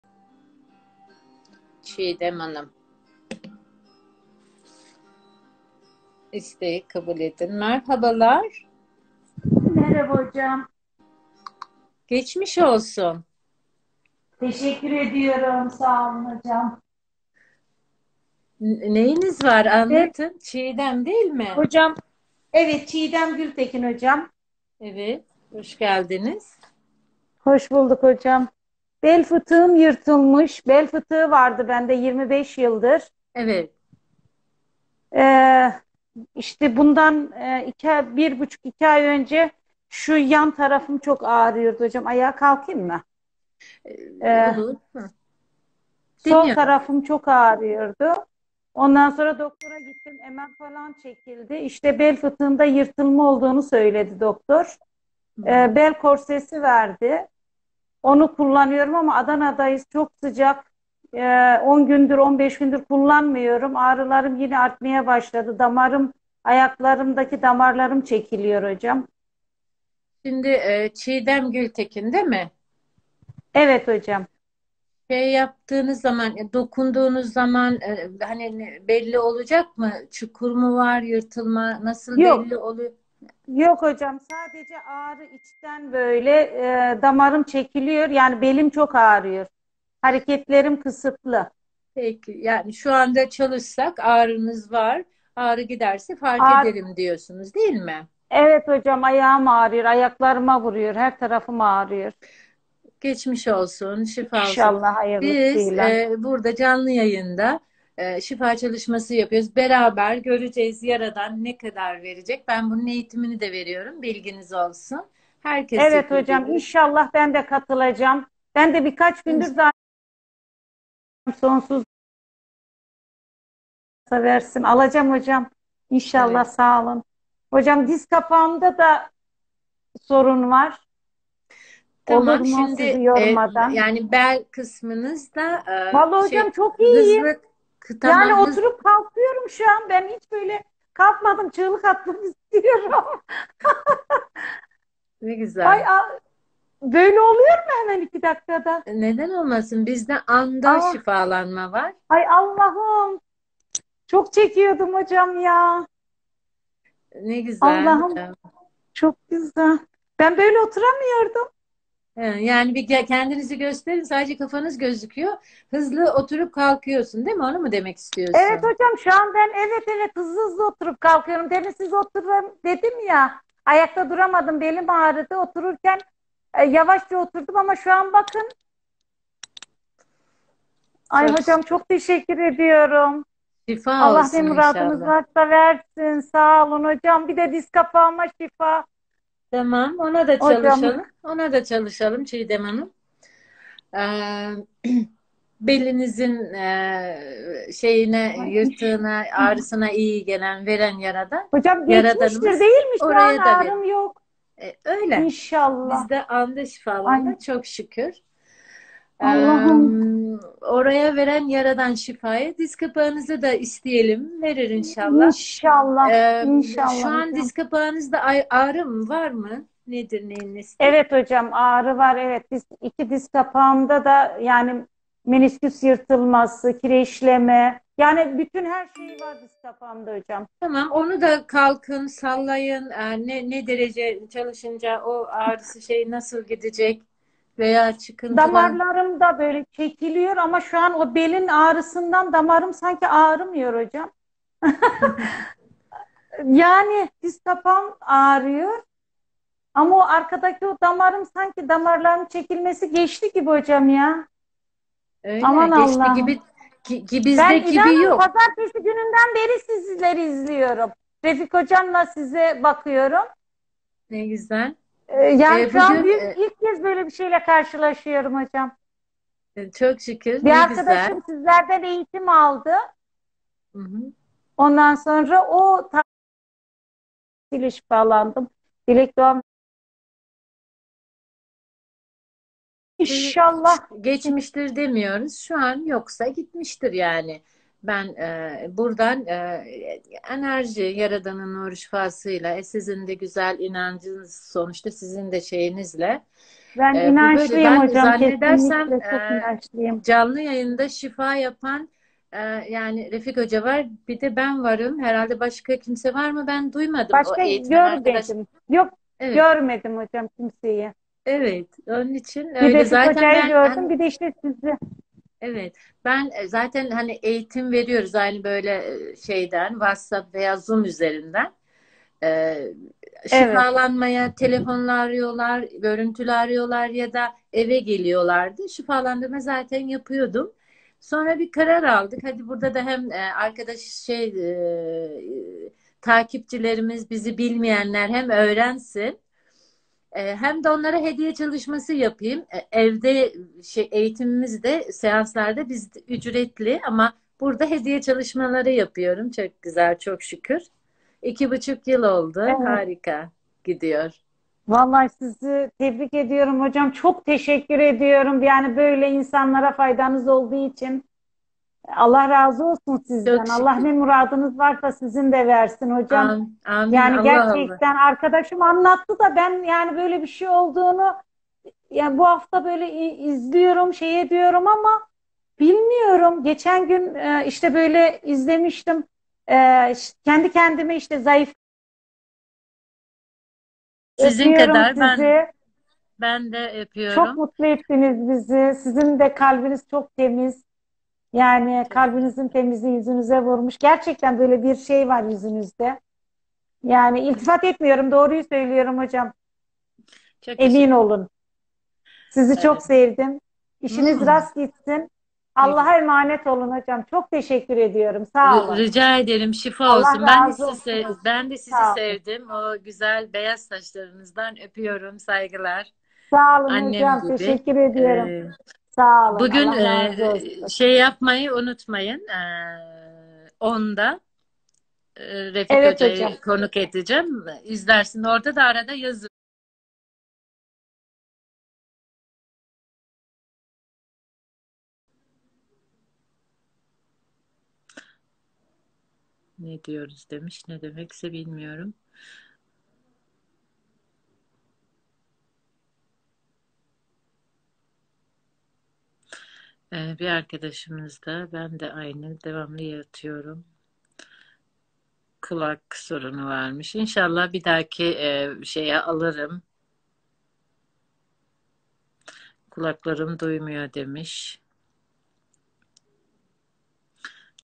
BEL ÇUKURU (LORDOZ) Dua ve ENERJİ ÇALIŞMASI (69)
Bek ve bacak ağrısı (Enerji frekans Çalışması) YETER Kİ İSTEYİN ÖĞRETİYORUZ